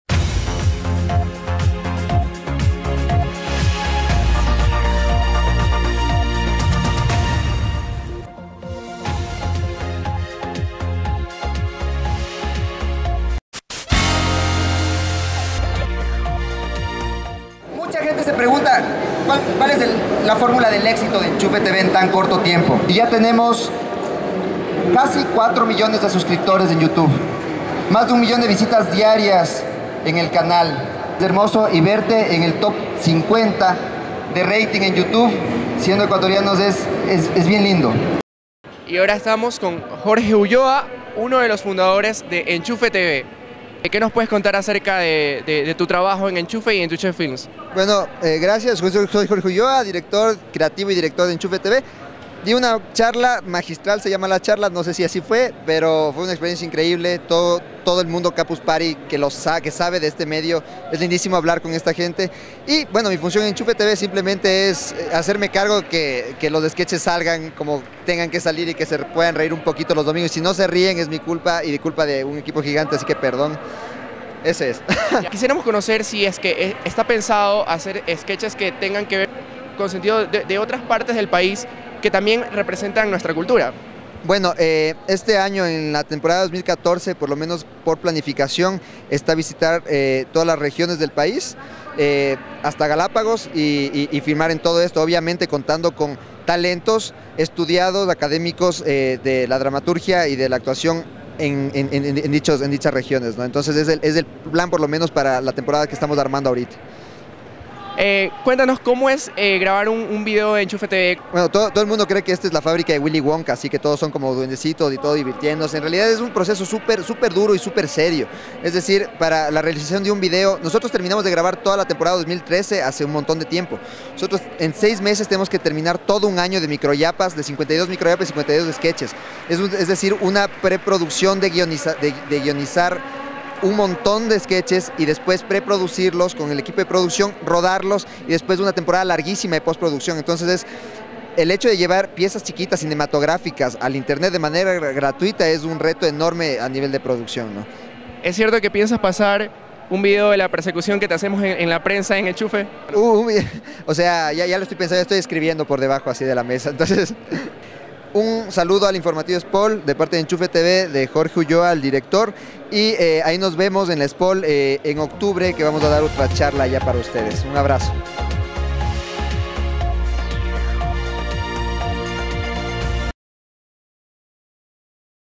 Enchufe tv en Campus Party Quito 2013
Los fundadores de enchufe tv dictaron una charla magistral en el campus party teniendo una gran acogida por los campuseros.